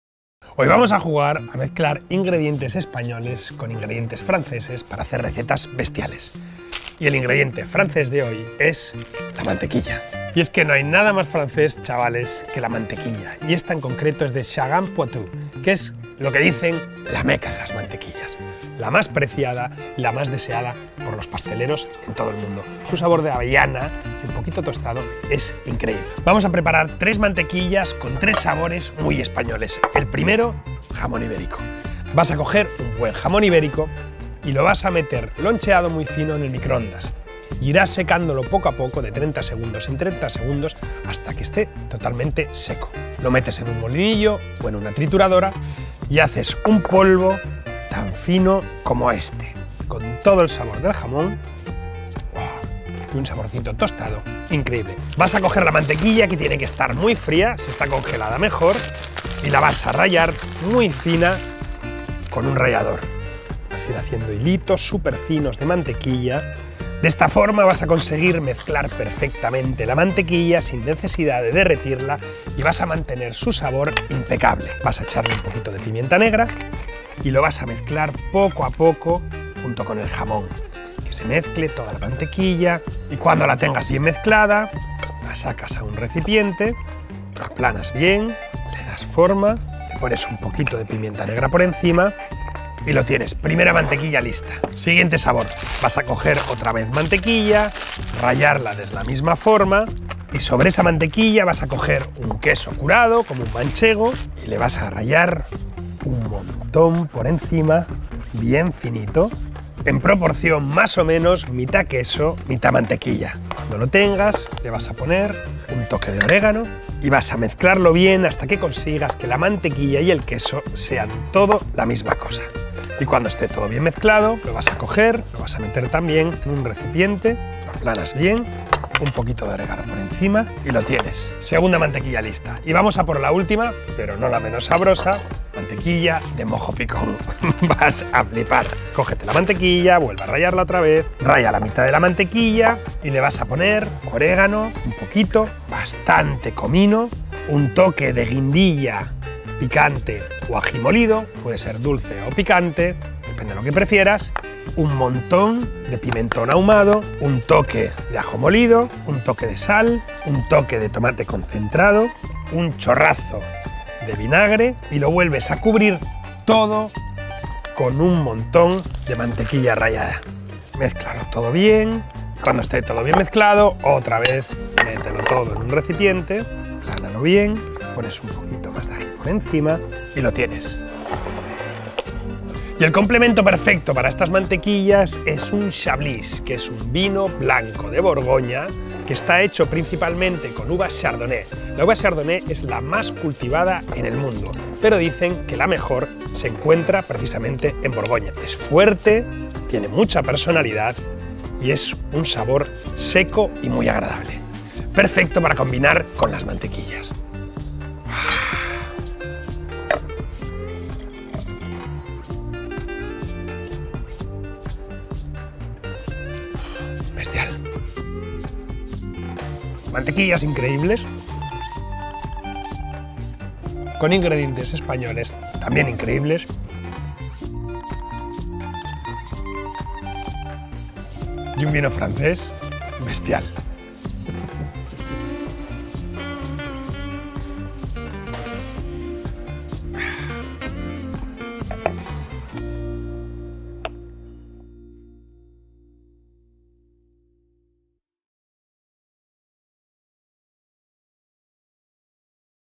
MANTEQUILLAS DE IBÉRICO, IDIAZÁBAL Y MOJO PICÓN | Gipsy Chef
Ahí va el último recetón de la serie Mix & Match Challenge de @tastefrancemagazine para el Ministerio de Agricultura de Francia.

¿Hay algo más francés que la mantequilla? Probablemente, lo único más francés que hay son: los franceses y todo aquello con lo que cocinan.

Vamos a juntar esta maravilla de la naturaleza con productos muy españoles. Atentos, lo que viene es bestial!!!